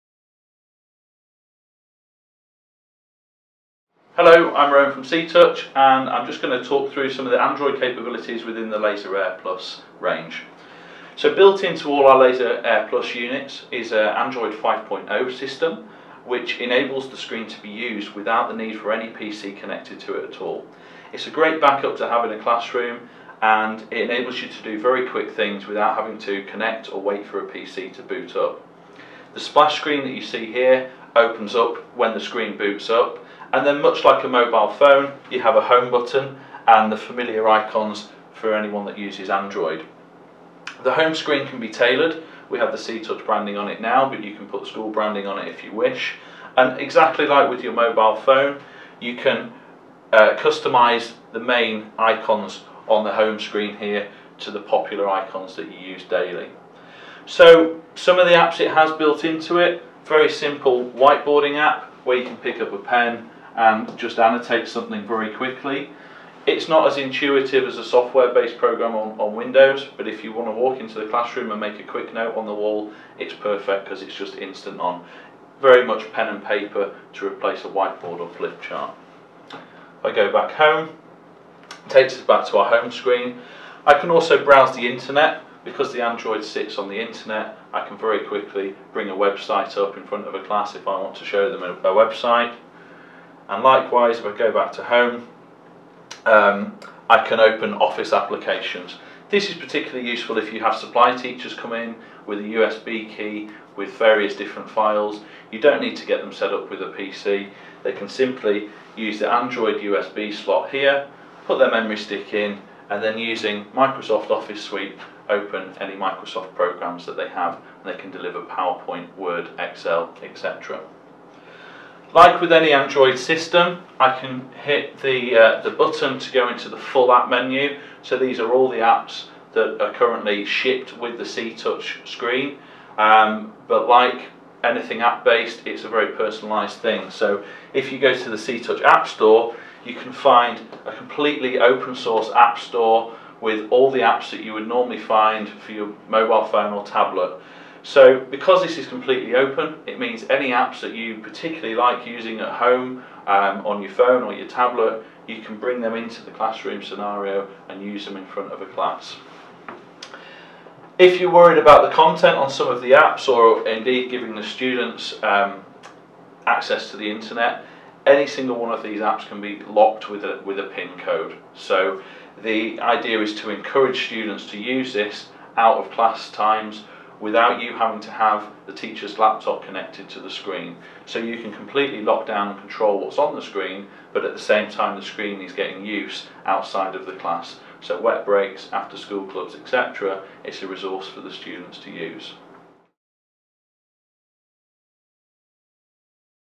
CTOUCH Built in Android Functionality
Explore the features of the built in Android solution in the CTOUCH Interactive touch displays for your classroom with Virtue Technologies